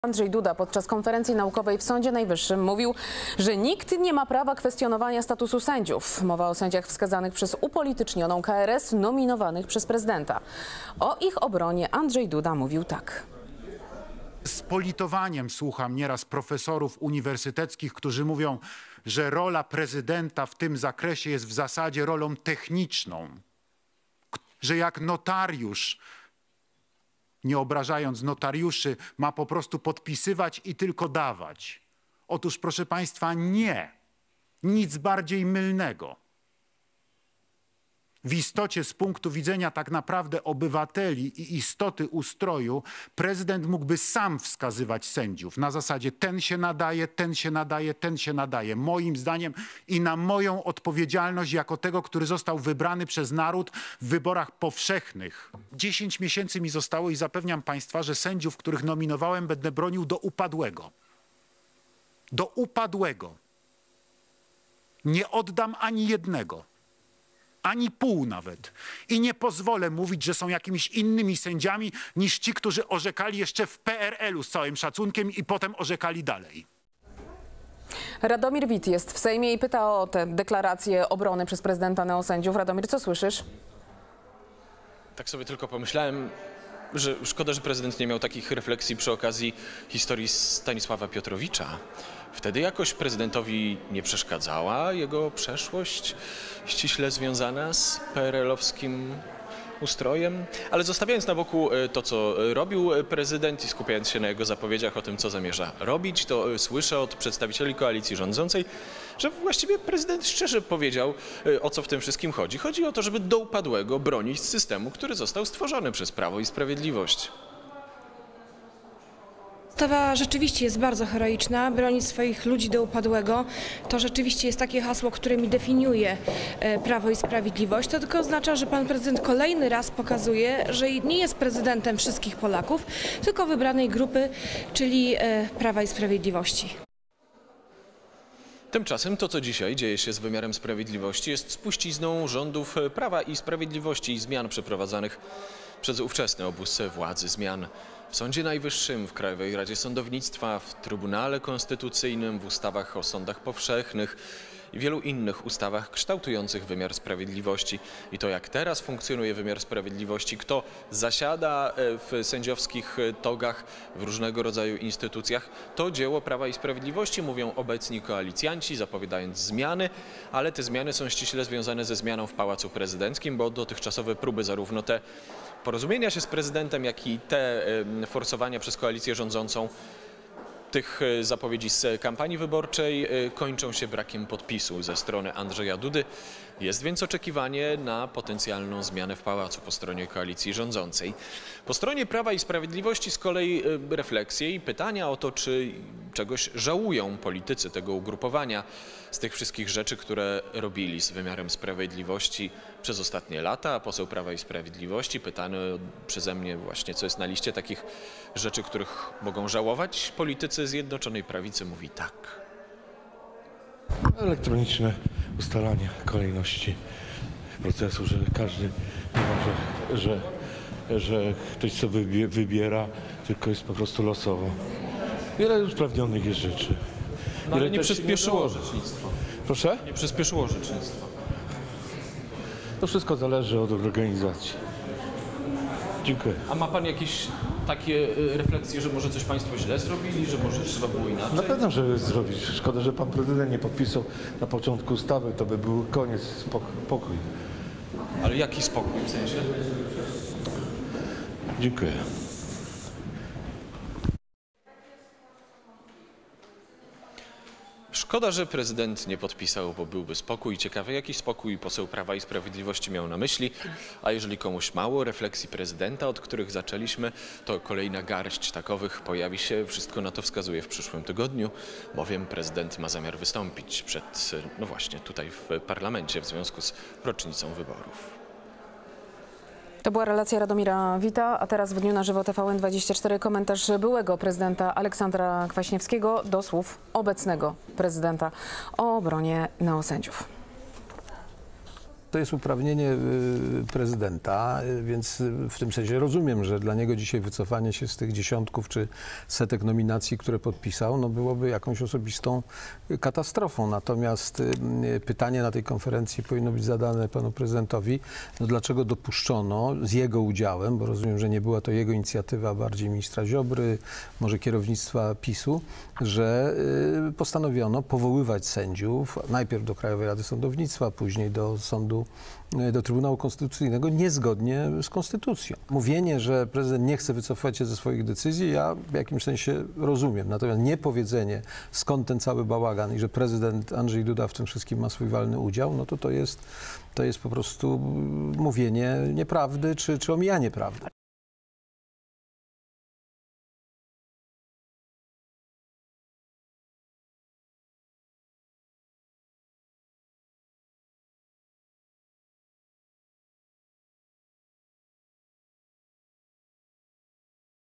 Prezydent broni neosędziów. Komentarze polityków po wystąpieniu Andrzeja Dudy
Podczas konferencji naukowej w Sądzie Najwyższym prezydent Andrzej Duda mówił, że nikt nie ma prawa kwestionować statusu sędziów - Dziesięć miesięcy mi zostało i zapewniam, że sędziów, których nominowałem, będę bronił do upadłego. Nie oddam ani jednego, ani pół - przekonywał. Wypowiedź prezydenta komentował Radomir Wit z Sejmu - Szkoda, że prezydent nie miał takich refleksji przy okazji historii Stanisława Piotrowicza. Wtedy jakoś prezydentowi nie przeszkadzała jego przeszłość ściśle związana z PRL-owskim ustrojem - powiedział Wit.